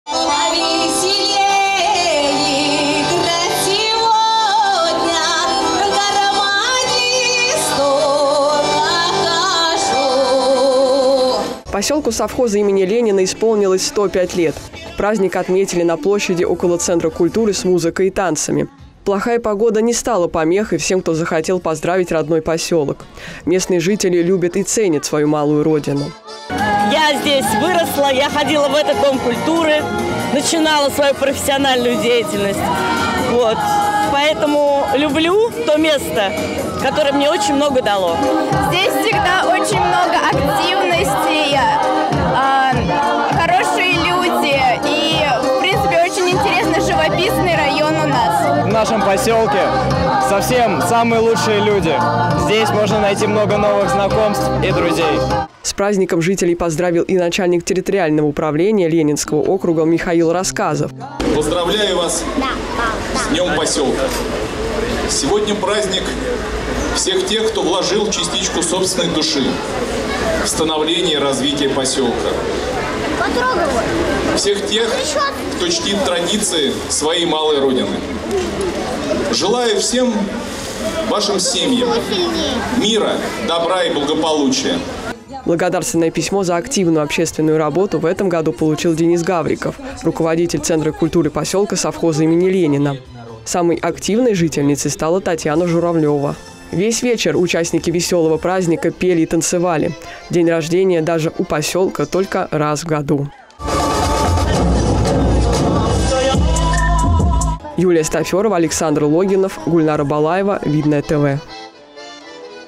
ДЕНЬ ПОСЁЛКА
Поселку Совхоза имени Ленина исполнилось 105 лет. День рождения отметили массовым гуляньем. Обо всех подробностях расскажет наш корреспондент Юлия Стафёрова.